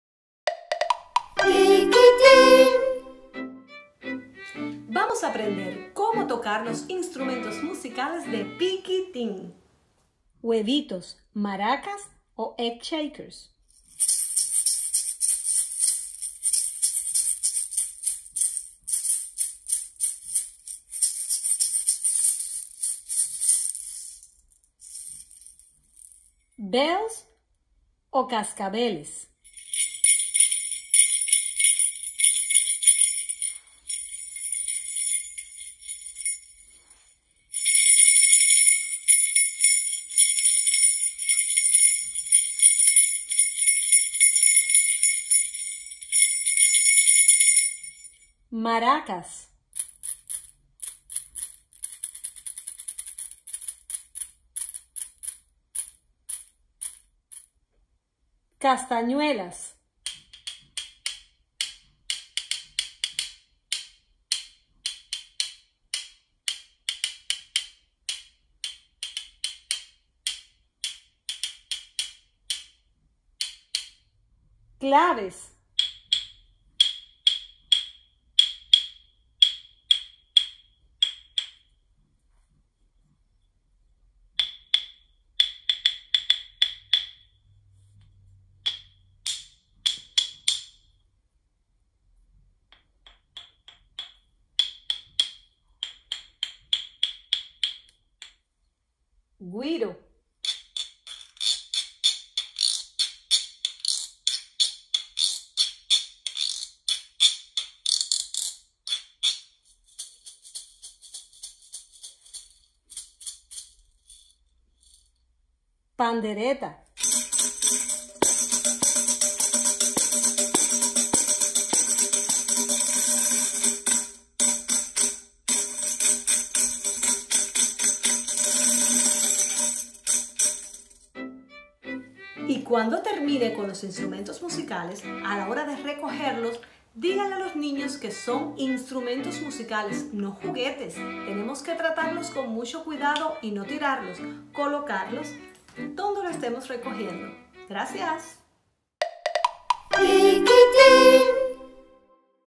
Musical Instruments: Instrumentos musicales
How to use Pikitin musical instruments. Como usar los instrumentos musicales de Pikitin.